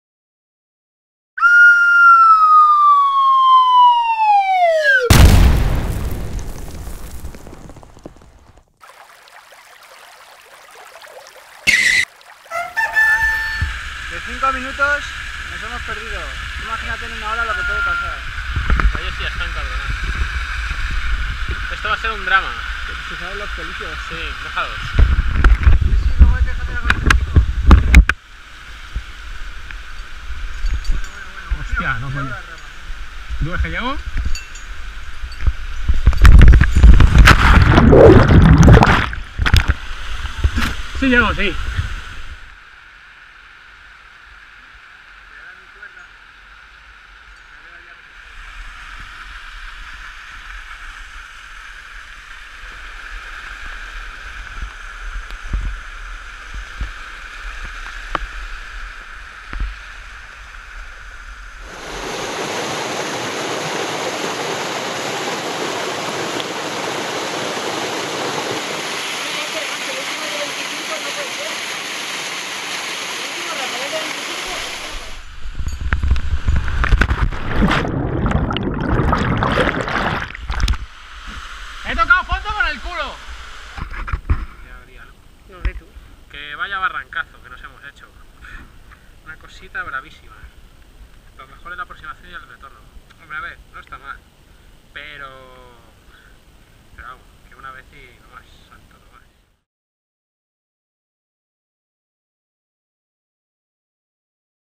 Barranco del Frades Superior (12-07-14)
Buen barranquito en la sierra da Freita portuguesa, es decir, allá donde Cristo dió las Tres Voces. Aproximación bastante amable, una vez que adivinas por donde es (nos costó mas de tres minutos encontrarla, y un par de jareos locos). Para el que se acerque: dejas el coche en el Cementerio de Cabreiros, subes a Tebilhao (dejas las cosas, bajas el coche, subes andando, son 2 km) desde la entrada de Tebilhao por la carretera (no por el PR) sale una carreterilla-pistilla: bueno, pues tiras por ahí, y cuando veas una valla de hormigón con una cancela de madera, entras. Bajas siguiendo el cercado de postes de hormigón a través de los helechos, y sigues bajando, sigues... y entras al río. Desde aqui, vas pasando saltos, resaltes y un rapel chulo fraccionado. Eso si, te vas a hichar a andar por el río resbaloso.

Como tuvimos un problemilla con un par de escarpines, me hice el barranco con calcetos, y pillé lo mío. Tanto que cuando llegamos al puente que marca el fin del barranco, Dani y Jessi se hicieron el inferior y Bea y yo retornamos al pueblo. Tomadlo con calma que el PR es un cuestón de los buenos... y de 20 minutos de retorno, nada (Unos 40). Aún así un barranco molón ... aunque de esos que no repites :D

Con Daniel Valadés Diaz, Jéssica Manzano y Bea Crespo